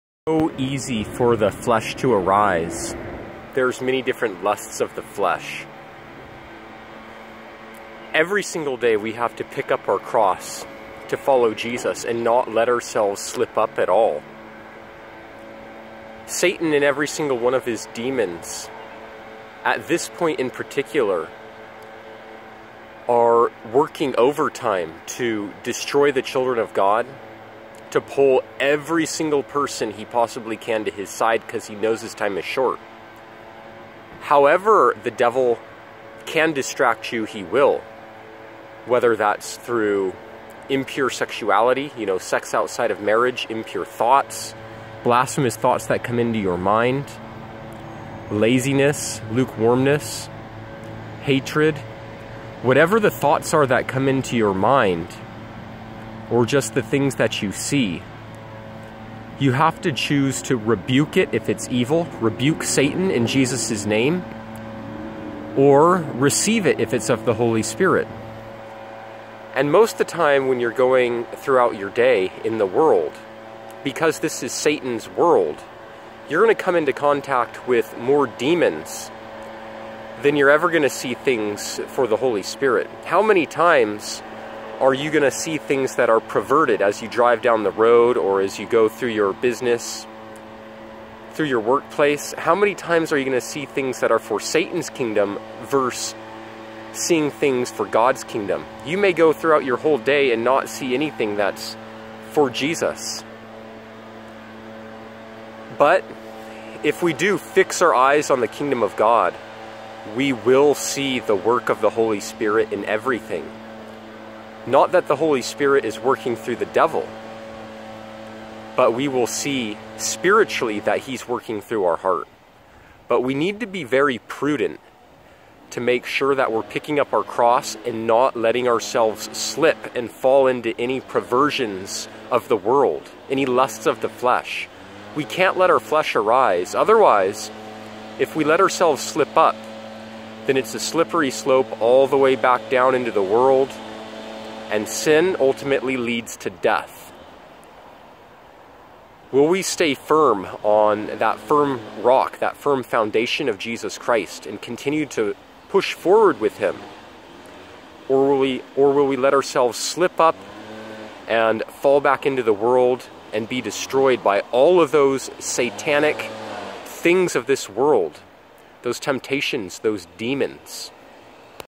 Don't Let Yourself Slip Up and Fall
It’s so easy for the flesh to arise, there are many different lusts of the flesh.  Every single day we have to pick up our cross to follow Jesus and not let ourselves slip up at all.  satan and every single one of his demons at this point in particular are working overtime to destroy the children of God, to pull every single person he possibly can to his side because he knows his time is short.  However the devil can distract you he will, whether that’s through impure sexuality, you know, sex  outside of marriage, impure thoughts, blasphemous thoughts that come into your mind, laziness, lukewarmness, hatred, whatever the thoughts are that come into your mind or just the things that you see, you have to choose to rebuke it if it’s evil, rebuke satan in Jesus’s name, or receive it IF IT’S OF THE HOLY SPIRIT.  And most of the time when you are going throughout your day in the world, because this is satan’s world, you are going to come into contact with more demons, than you are ever going to see things for the Holy Spirit.  How many times are you going to see things that are perverted as you drive down the road or as you go through your business, through your workplace? How many times are you going to see thing that are for satan’s kingdom verse seeing things for God’s kingdom?  You may go throughout your whole day and not see anything that’s for Jesus.  But, if we do fix our eyes on the Kingdom of God, we WILL see the work of the HOLY SPIRIT in EVERYTHING.  Not that the Holy Spirit is working through the devil, but we will see spiritually that He is working through our HEART.  But we need to be very prudent that we are picking up our cross and not letting ourselves SLIP and fall into any of the perversions of the world, any lusts of the flesh, we can’t let our flesh arise, otherwise, if we let ourselves slip up, it’s a slippery slope all the way back down into the world, and sin ultimately leads to death.  Will we say firm on that firm rock, that firm foundation of Jesus Christ and continue to push forward with Him?  Or will we let ourselves slip up and fall back into the world and be destroyed by all of those satanic things of this world, those temptations, those demons?